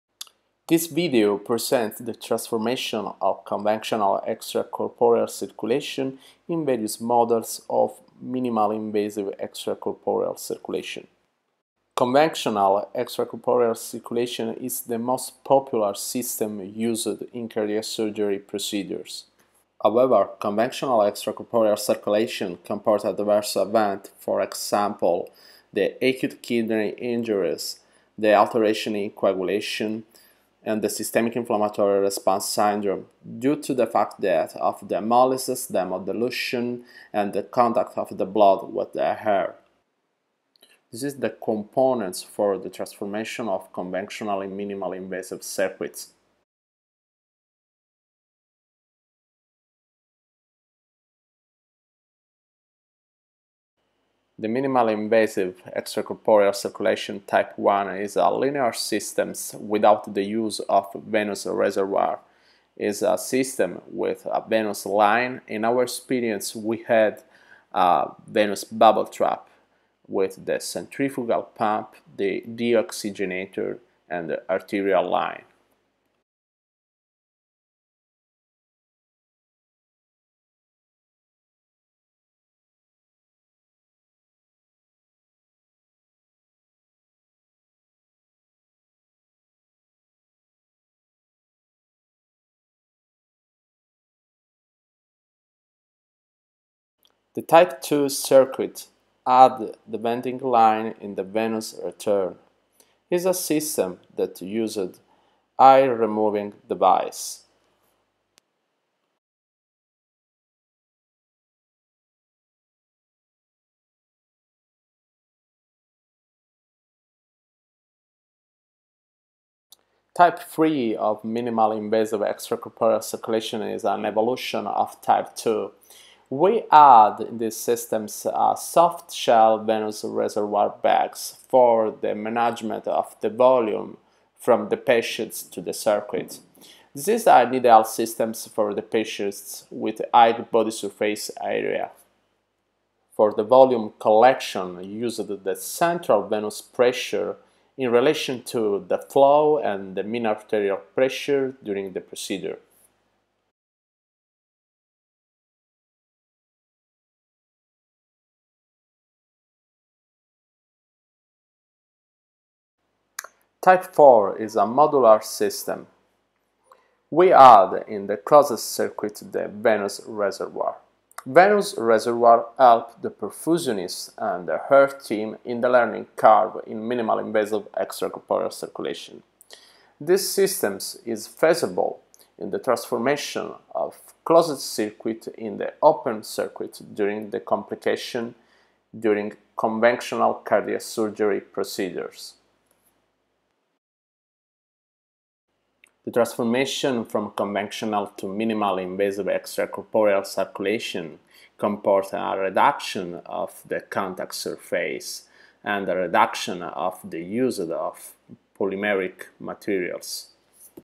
From Conventional Circuit to MiECC
This video shows the simple realization of minimally invasive extracorporeal circulation models, starting from a traditional system.